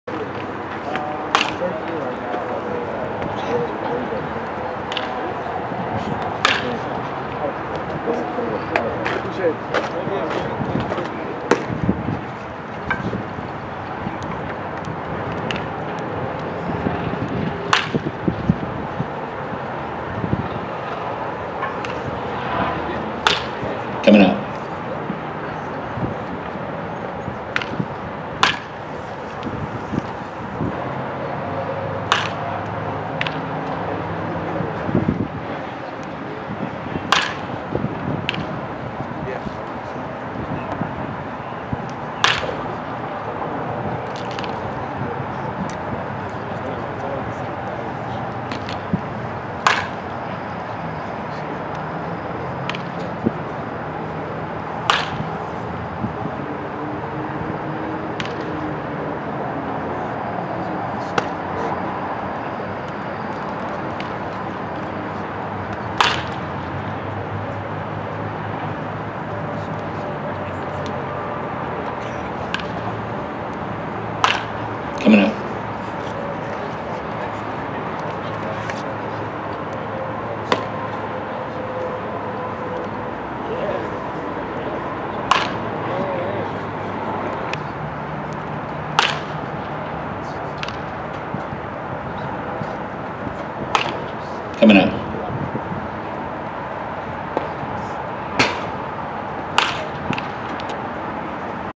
Maximus Martin, IF, Batting Practice
IF Maximus Martin takes batting practice at the 2021 Baseball Factory All-America Game at the Kansas City Urban Youth Academy.